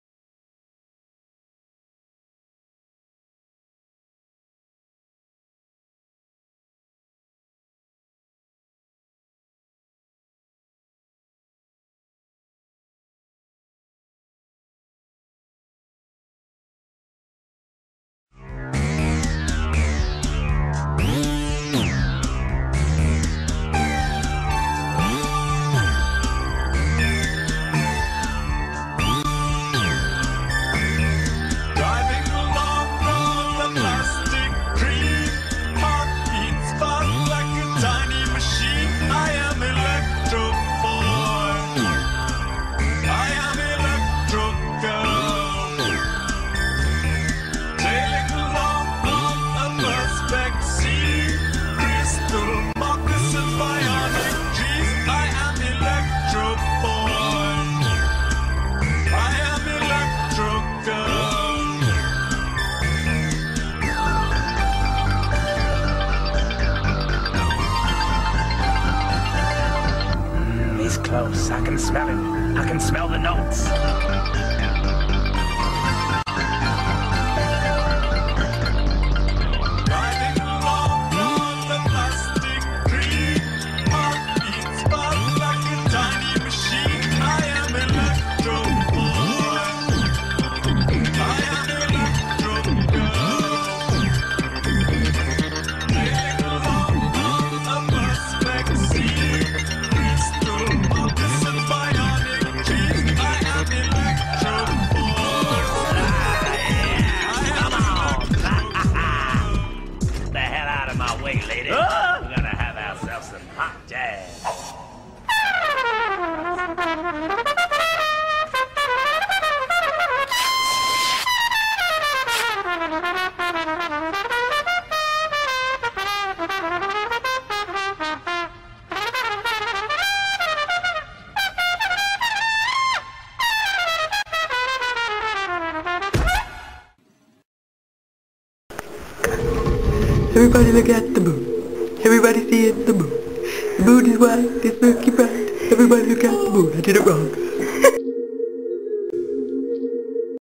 Electro Squares, Triangles and various other Shapes.
This is our entry to the Pull your Shapes competition sir, coming at you all the way from the Land Down Under. Together we pulled more shapes than a maths textbook eating a lego tower.
Direct your eyes, if you please, to the mighty boosh on the electrobot's head, made from the offcuts of a hobo gorilla's perm.
Enjoy our little video...we did.